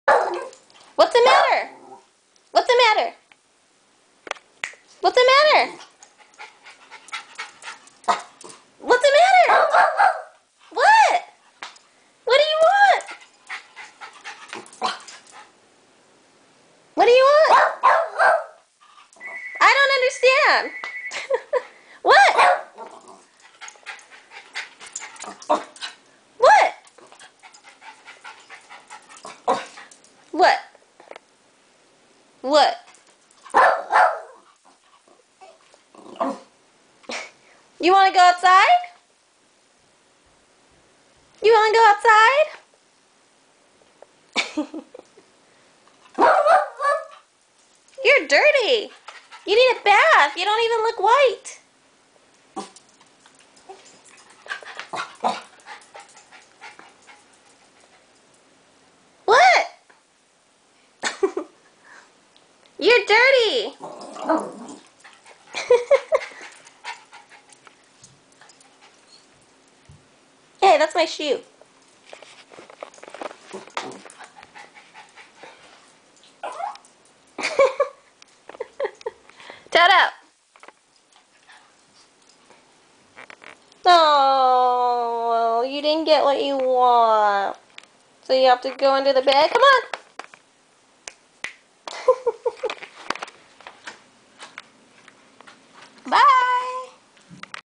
Talking Dog
Recording my dog while I was cleaning my room. He heard people outside and wanted to go out but I wouldn't let him so he is having a tantrum.